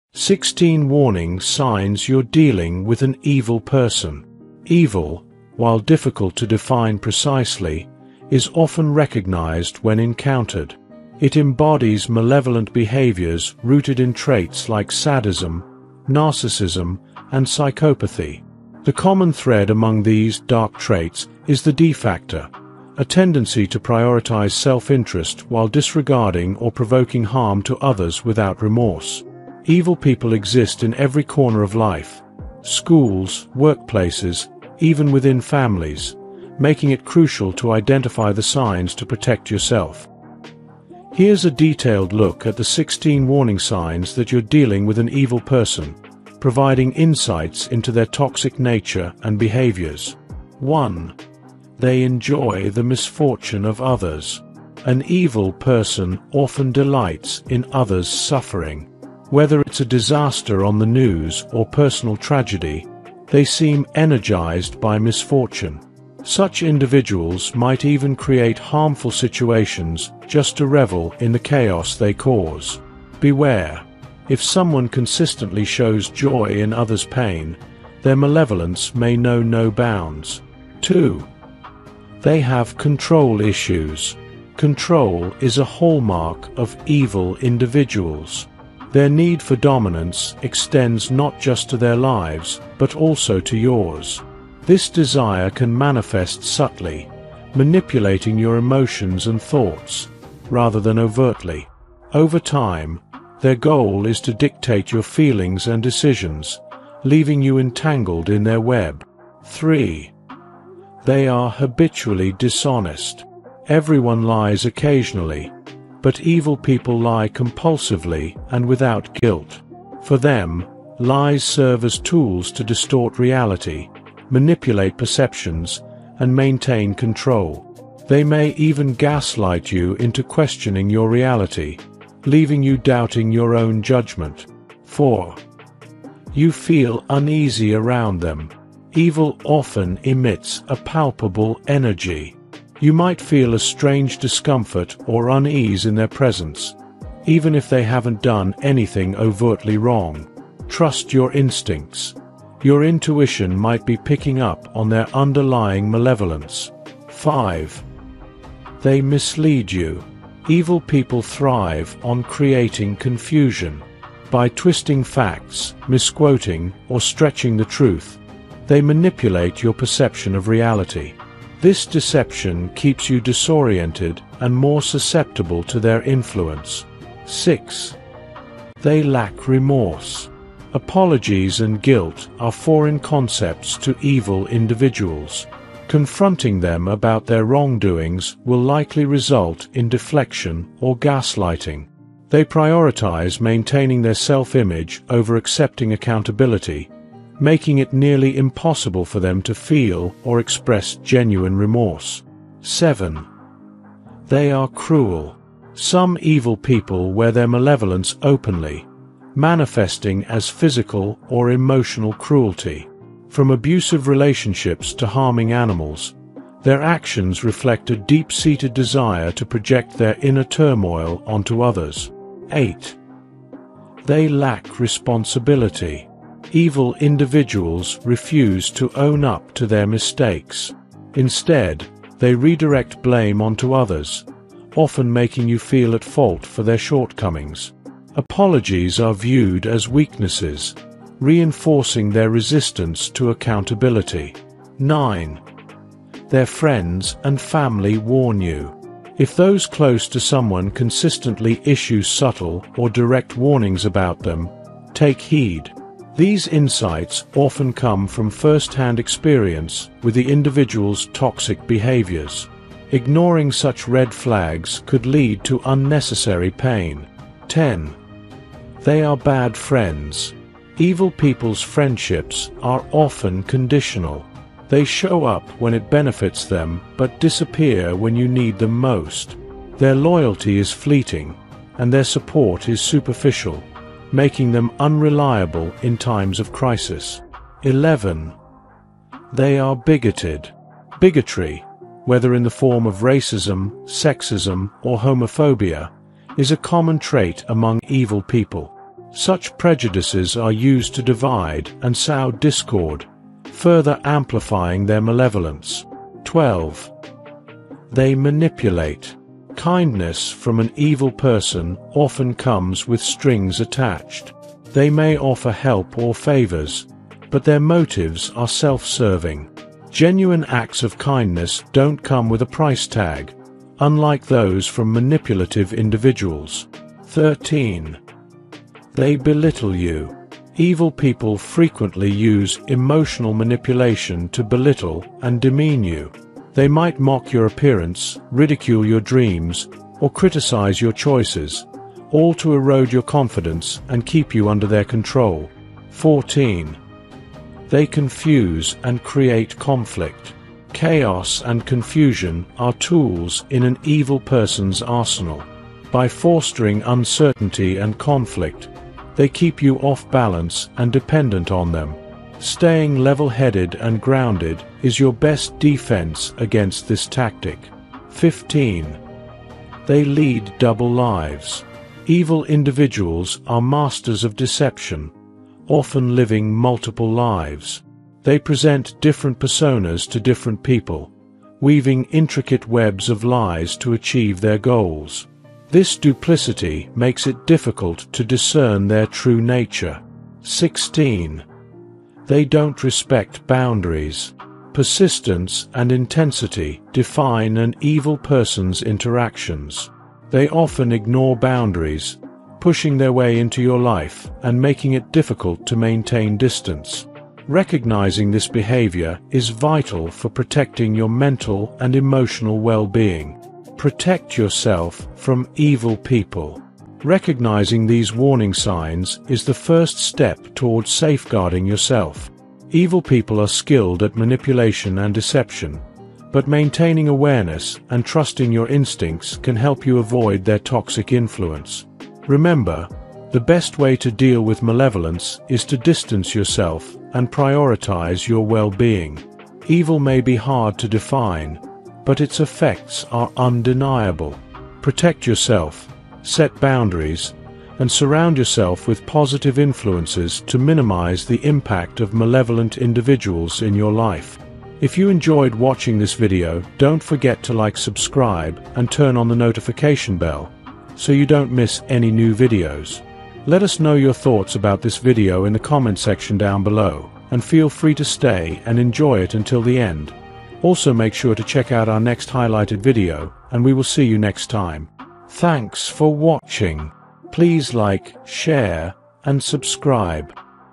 16 Warning Signs You're Dealing With an Evil Person | Warning Signs Of Evil People | Awesome Facts
16 Warning Signs You're Dealing With an Evil Person | Warning Signs Of Evil People | Awesome Facts
Are you wondering how to spot evil people and protect yourself from their harmful influence? In this video, we’ll explore 7 warning signs that can help you identify toxic, manipulative, and malevolent individuals before they negatively impact your life. Whether it’s in relationships, at work, or even within your social circle, understanding these signs is crucial to safeguarding your emotional and mental well-being.

What You’ll Learn in This Video:
The traits of manipulative people – Learn how to spot dishonesty and control tactics.
Emotional manipulation explained – Discover how they exploit feelings to gain power.
Recognizing narcissistic tendencies – Understand the signs of self-centered individuals.
How to identify lack of empathy – See the subtle ways cruelty shows up in behavior.
Energy intuition and red flags – Trust your gut when dealing with harmful personalities.
Why setting boundaries is vital – Stop toxic people from taking advantage of you.
Steps to protect yourself – Practical tips to avoid getting trapped by their tactics.
Why This Video Matters:
Evil people often operate in plain sight, disguising their harmful traits under charm or deception. If you don’t know the warning signs, you risk falling victim to their emotional, mental, or even physical abuse. With this video, you’ll become equipped to recognize these traits early and take action before it’s too late.

Who Should Watch This Video:
Anyone dealing with toxic people in their life.
People in relationships where something feels "off."
Those who want to improve their ability to read people.
Viewers interested in psychology, emotional intelligence, and self-protection.
More Helpful Resources:
How to Set Boundaries: [Insert Link]
Signs of Narcissistic Abuse: [Insert Link]
Understanding Emotional Manipulation: [Insert Link]